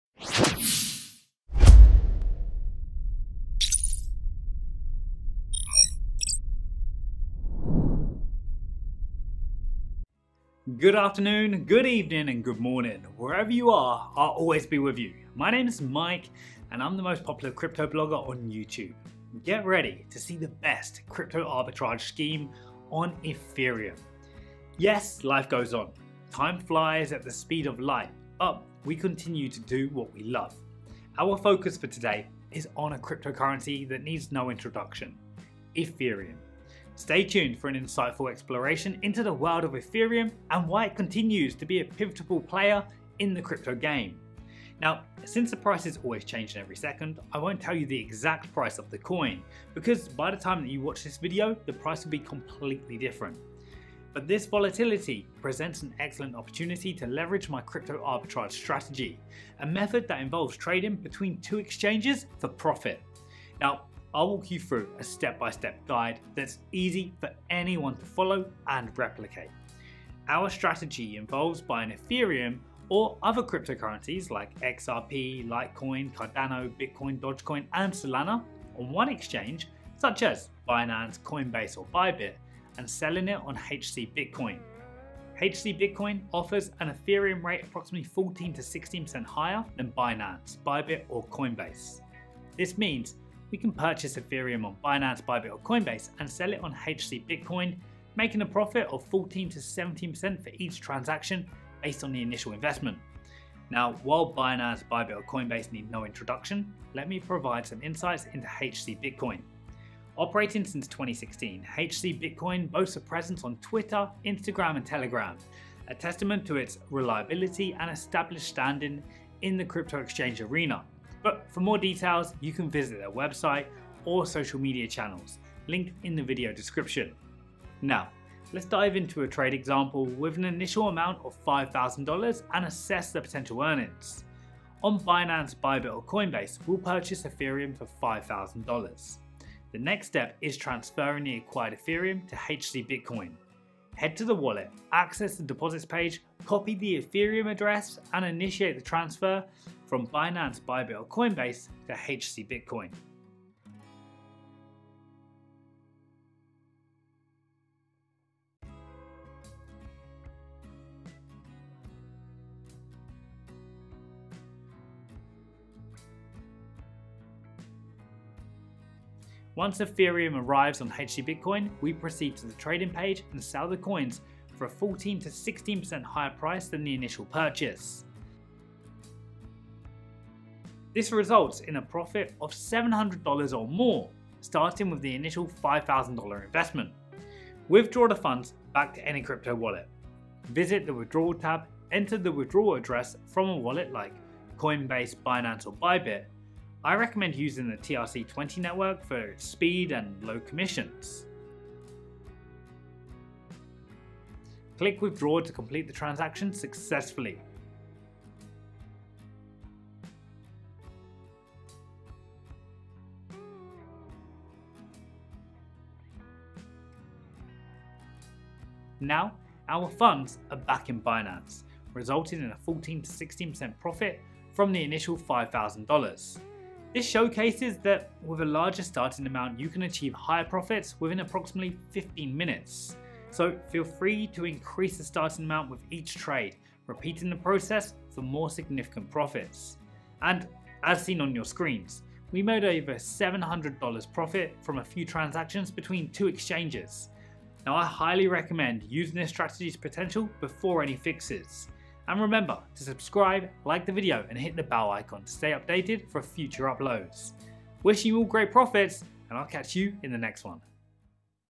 [Eth Crypto Method]Ethereum Arbitrage Strategy/Top Alt In 2024[Arbitrage Crypto]
It's your host Mike here. Today, I'm going to walk you through another example of coin arbitrage between two platforms. Enjoy the watch! :)

Sell on HcBitcoin (35 ETH without KYC)